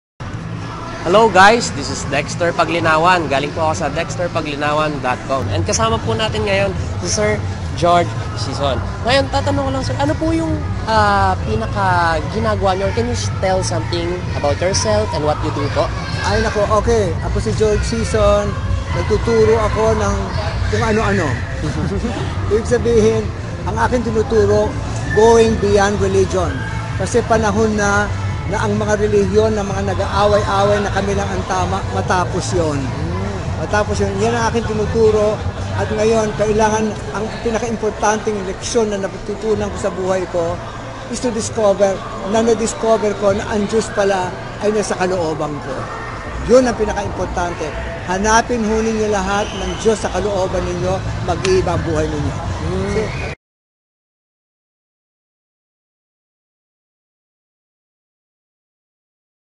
Dexter Paglinawan Interviews George Sison
| Philippines' Copywriter for the #1's and in-print master persuader Dexter Paglinawan interviews philosopher, author and public figure George Sison about 'favorite life lesson'.

٩(●̮̮̃•̃)۶